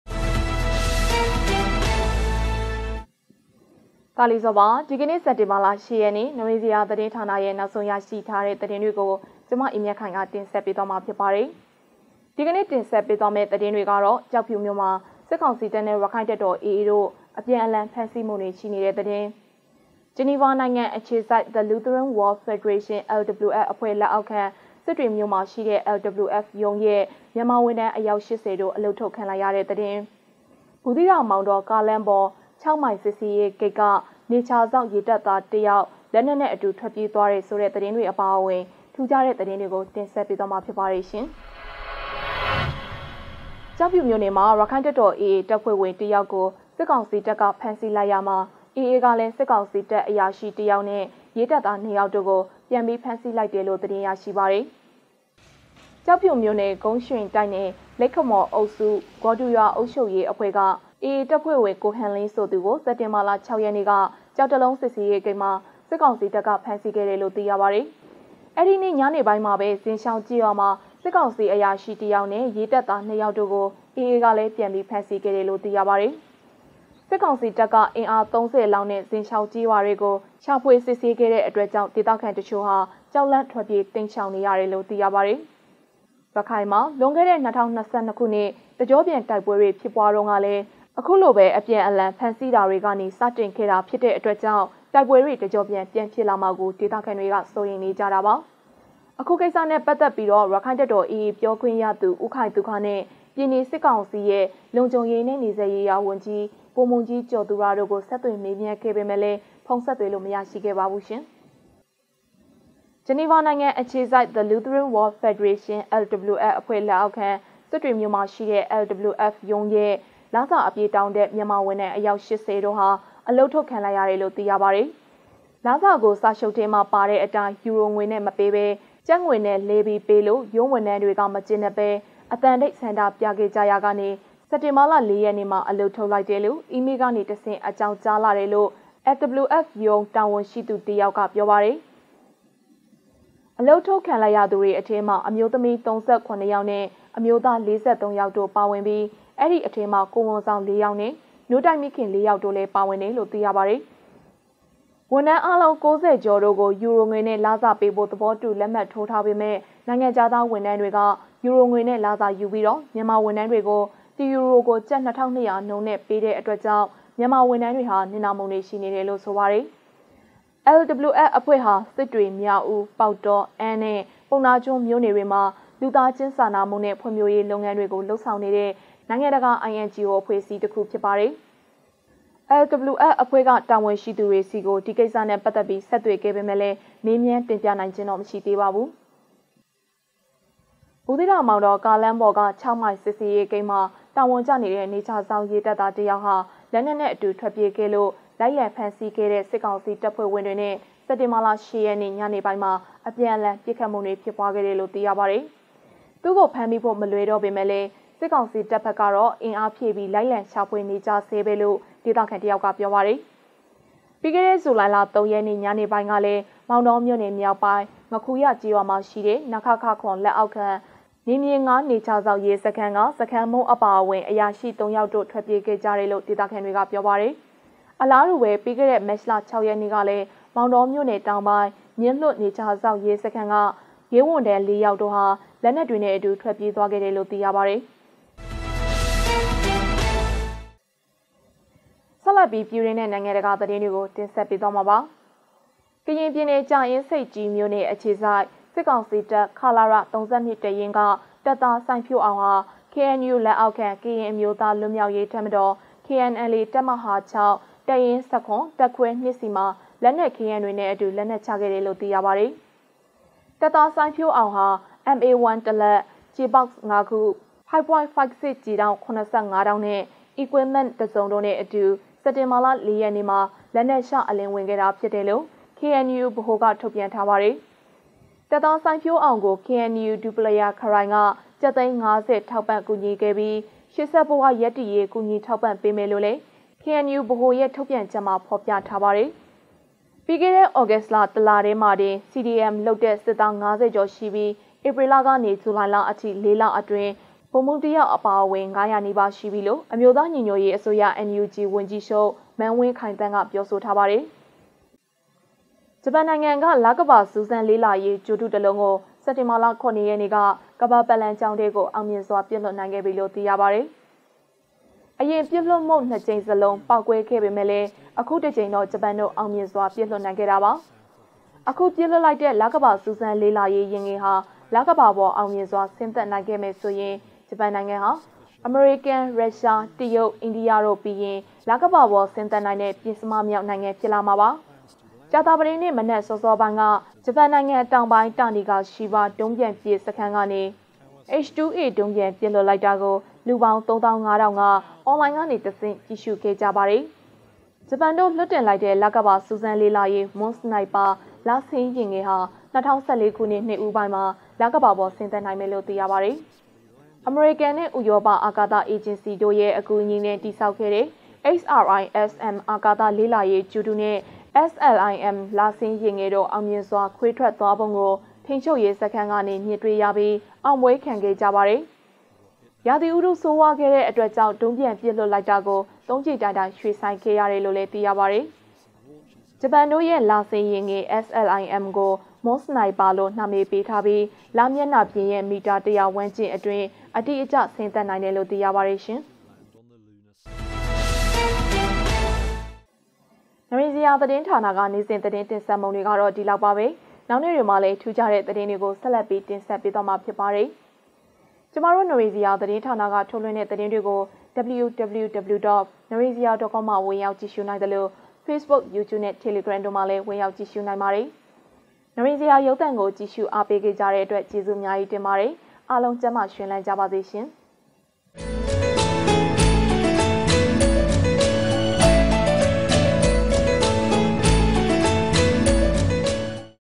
စက်တင်ဘာ ၈ ရက် နိရဉ္စရာရုပ်သံအစီအစဉ်
ဒီကနေ့ တင်ဆက်ပေးမယ့်သတင်းတွေကတော့

- ကျောက်ဖြူမြို့မှာ စစ်ကောင်စီတပ်နဲ့ ရက္ခိုင့်တပ်တော် (AA) တို့ အပြန်အလှန် ဖမ်းဆီးနေကြတဲ့ သတင်း

- ဂျနီဗာနိုင်ငံအခြေစိုက် The Lutheran World Federation (LWF) အဖွဲ့လက်အောက်ခံ စစ်တွေမြို့က LWF ရုံးရဲ့ မြန်မာဝန်ထမ်း အယောက် ၈၀ တို့ အလုပ်ထုတ်ခံလိုက်ရတဲ့သတင်း

- ဘူးသီးတောင်-မောင်တော ကားလမ်းပေါ် ၆ မိုင်စစ်ဆေးရေးဂိတ်က နယ်ခြားစောင့် ရဲတပ်သားတစ် ယောက် လက်နက်နဲ့အတူ ထွက်ပြေးခဲ့တဲ့သတင်း